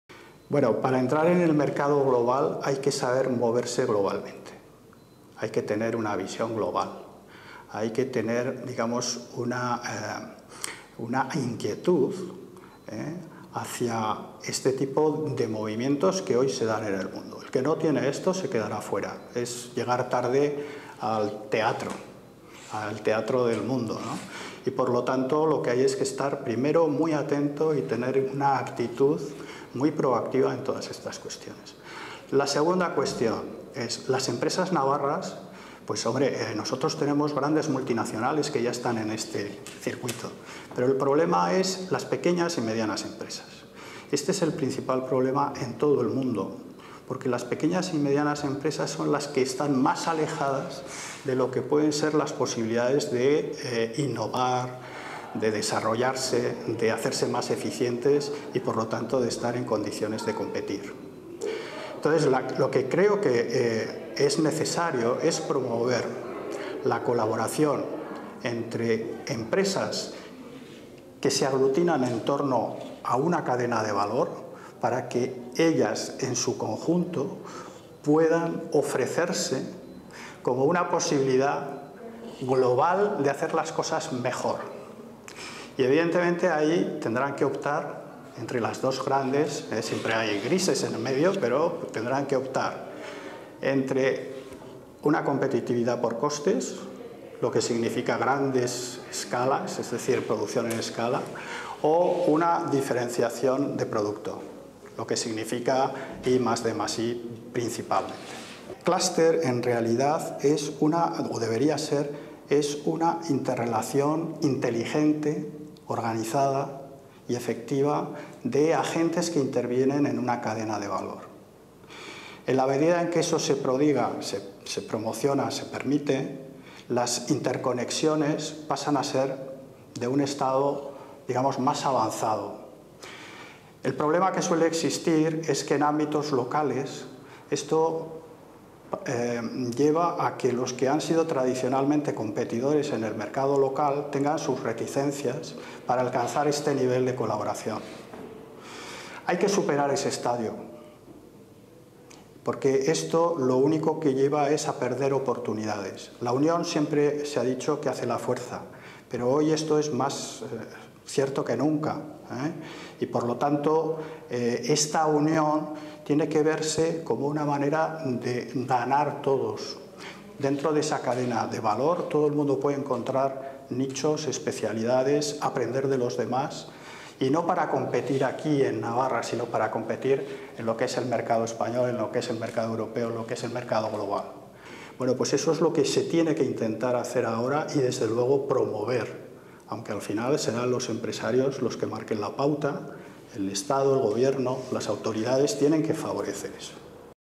Enrique Díaz. Cooperación empresarial
Jornada sobre cooperación empresarial: "¿Y si cooperamos para ser más fuertes? La cooperación empresarial como fórmula para mejorar el desarrollo de Navarra". Jornada sobre cooperación empresarial a cargo de Enrique Díaz, Socio Fundador de Businessfokus, experto en clústeres y competitividad.